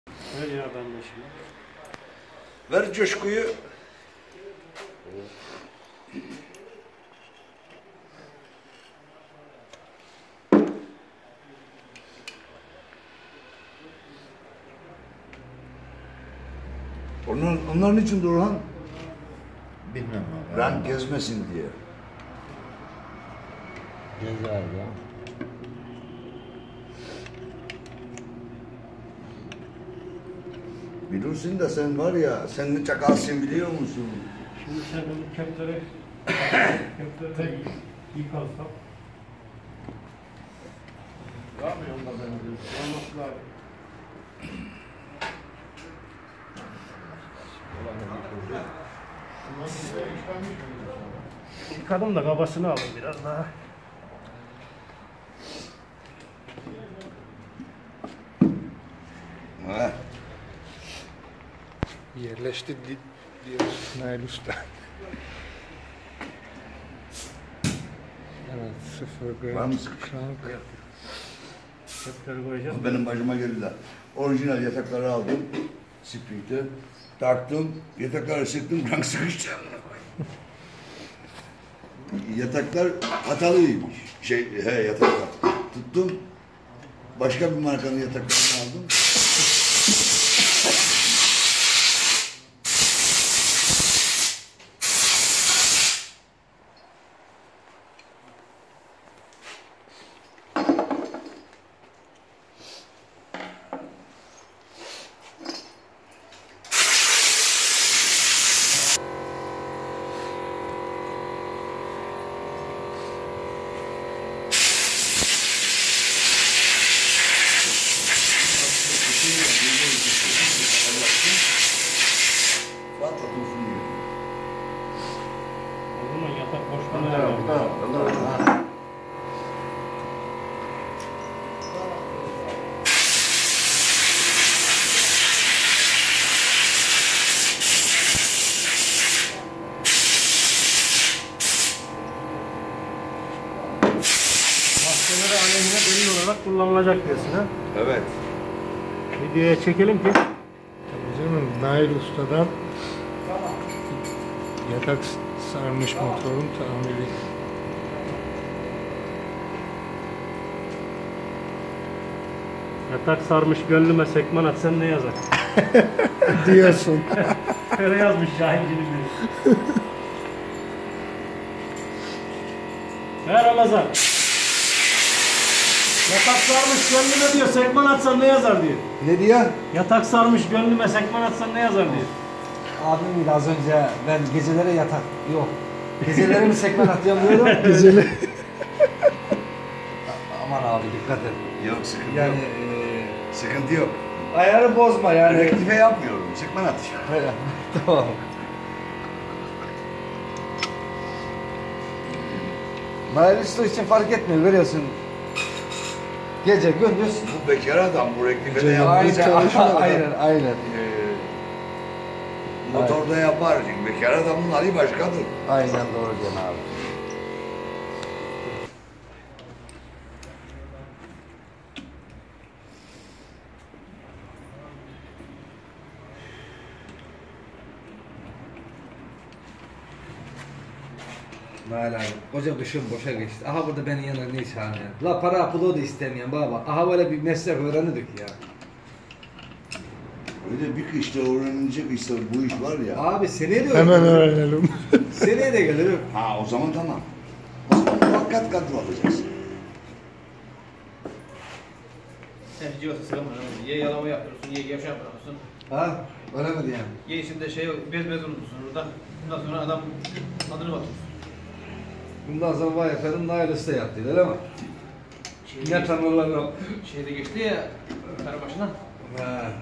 RİZE FINDIKLI'DA YATAK SARMIŞ RENAULT SPRİNG (NAİL USTA)(3)
YATAK SARAN RENAULT SPRING NAİL USTANIN ELİNDEN MOTOR SIFIR YAPILDI VE TEK MARŞTA ÇALIŞTI. RİZE FINDIKLI DA Kİ TÜM USTALAR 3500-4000TL ARASI İSTEDİLER  NAİL USTAM SADECE 1800TL YE MOTORU SAAT GİBİ YAPTI. HERKESE TAFSİYE EDERİM.